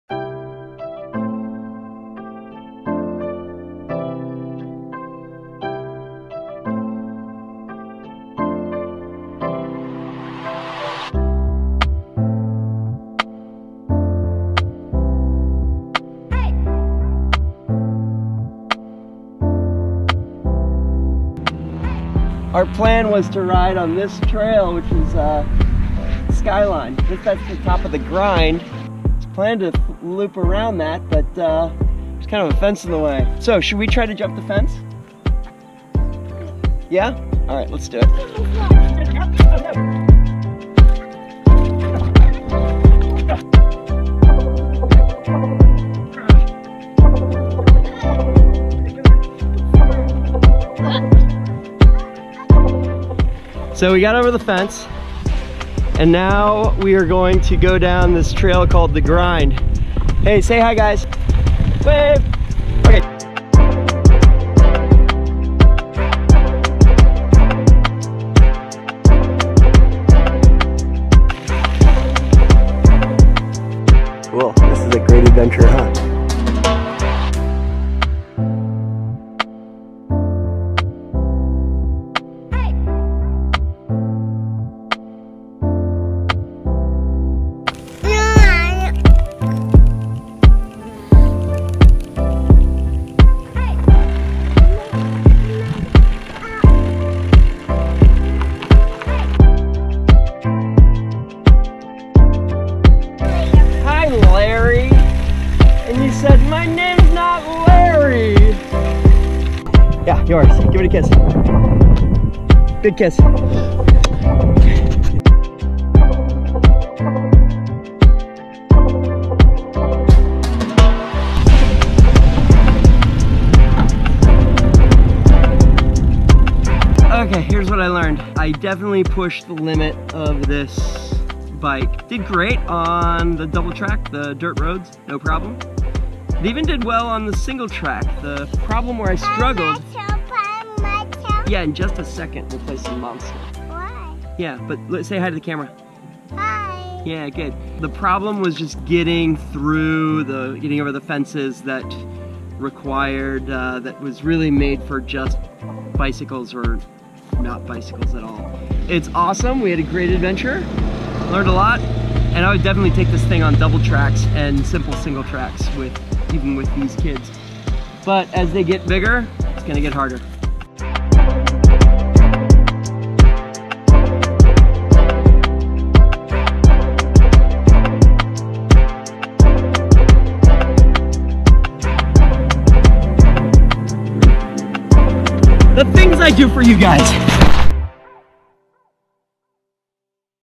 I took toddlers mountain biking on a Spicy Curry electric cargo bike
After two months of successful commuting with my kids on the Yuba Spicy Curry, I could no longer resist the urge to take them mountain biking. We stayed on mellow trails, but the hardest part was getting to the trail.